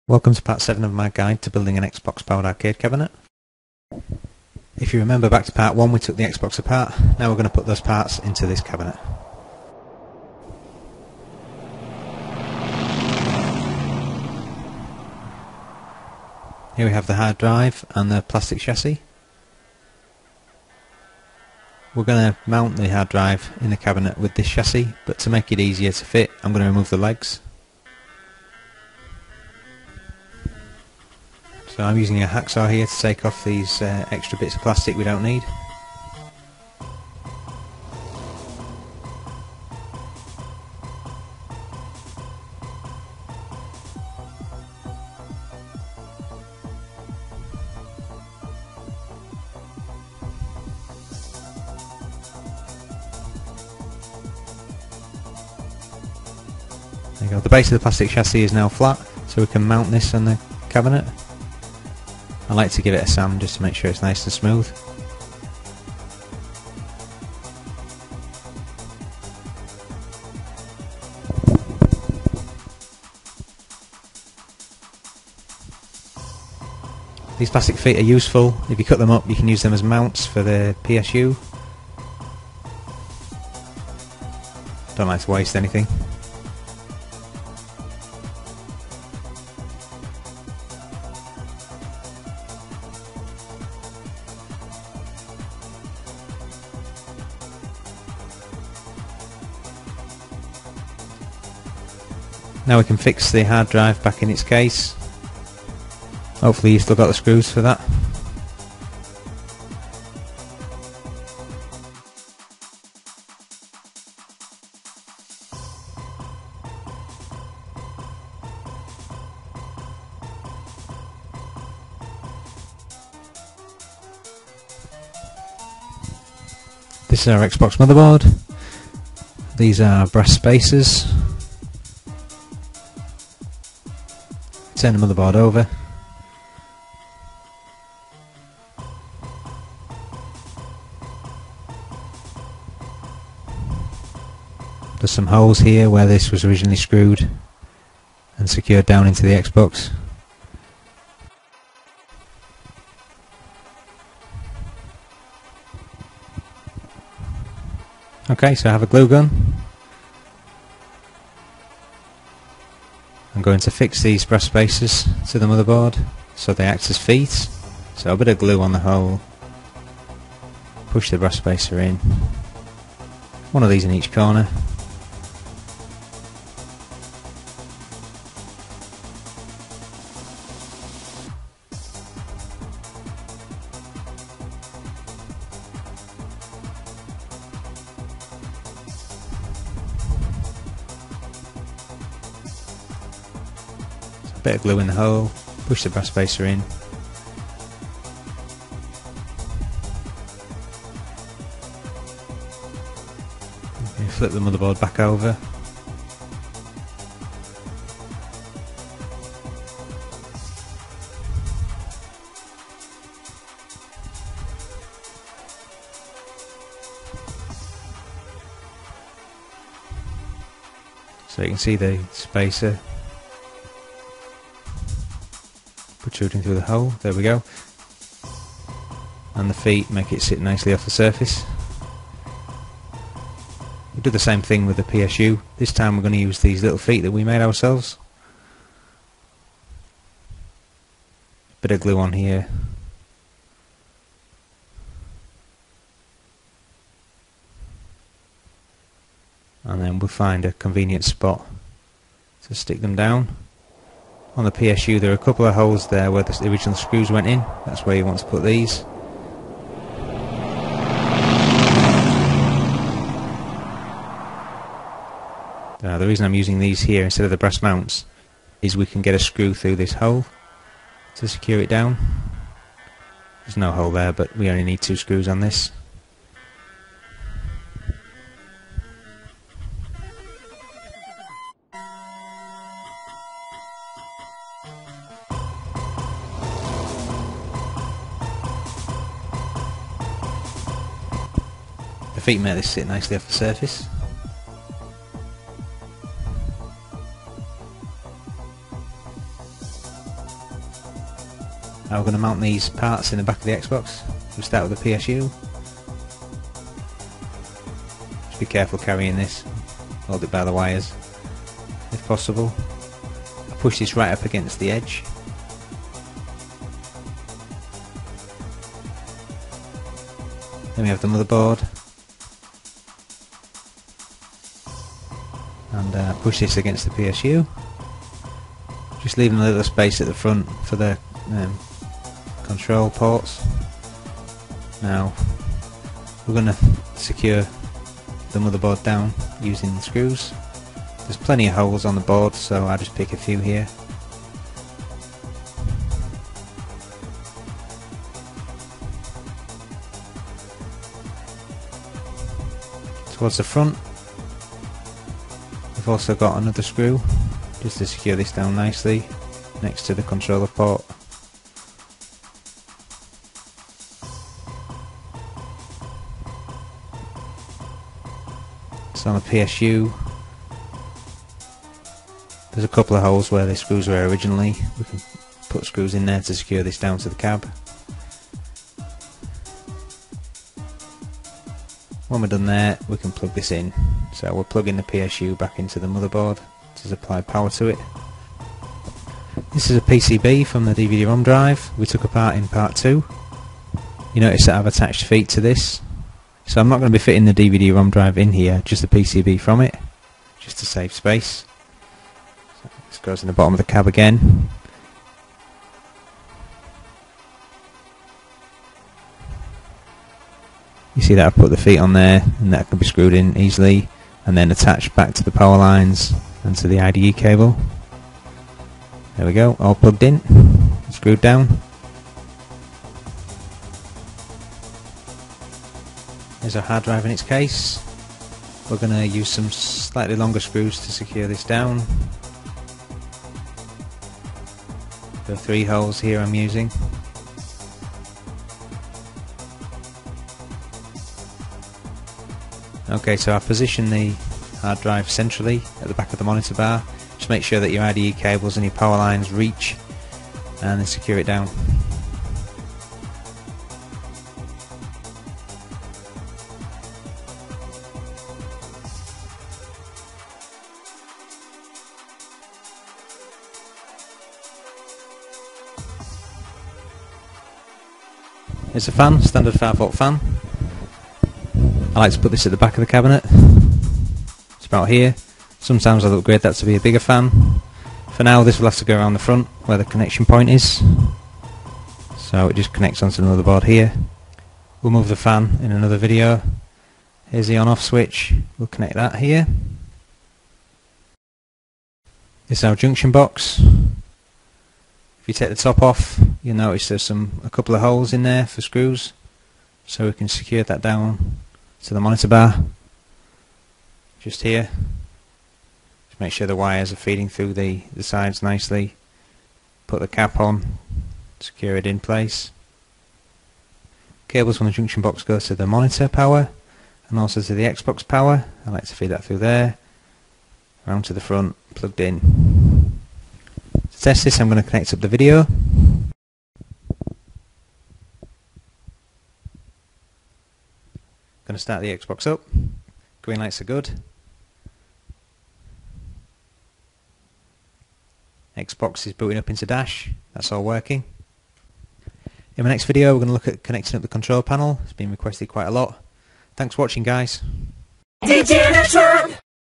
Building an xbox powered arcade cabinet part 7: Mounting the xbox parts
In this video we will fit the xbox motherboard, psu, hdd and other xbox parts into the cabinet.  These parts were removed from the xbox in my part 1 video.  Please refer back to that video if you have not yet disassembled the xbox.

In the video,  I fit the fan to the front of the cabinet.  I will later move this to the rear and it will be mounted in the rear panel.

This is a fairly straighforward part and no further explanation is really necessary.

In my next video (part 8) I will fit connect the control panel to the xbox.  This feature has been requested a lot.  I will also connect up an arcade button to control the xbox power - by soldering a wire to the on/off board.